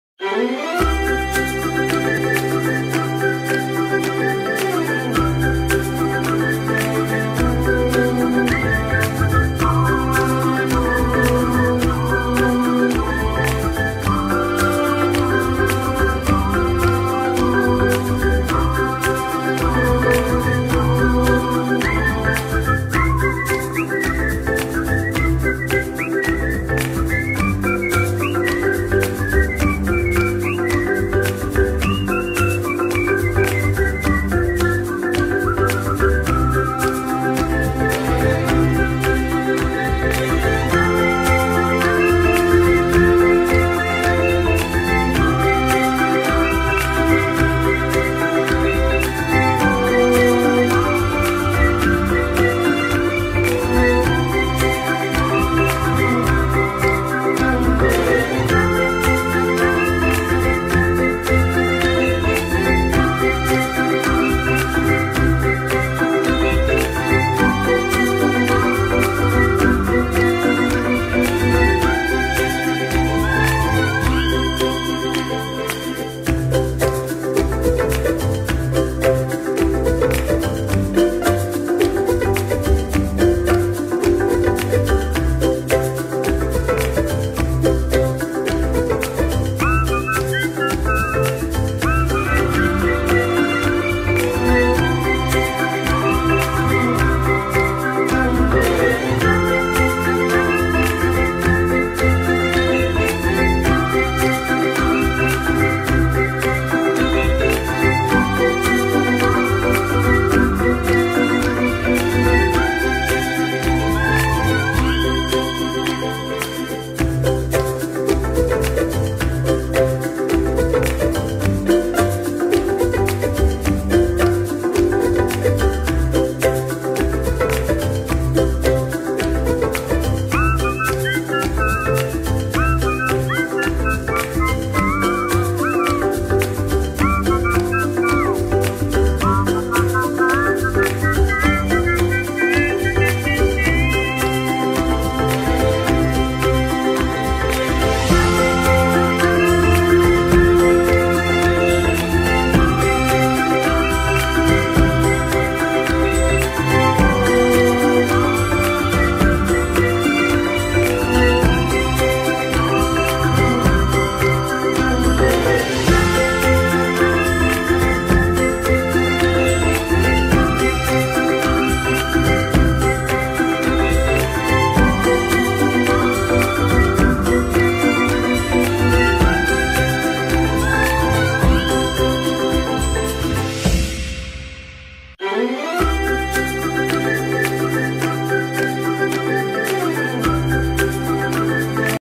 Si vous avez des pommes de terre et de la viande à la maison, surprenez tout le monde avec cette
Si vous avez des pommes de terre et de la viande à la maison, surprenez tout le monde avec cette recette facile et délicieuse !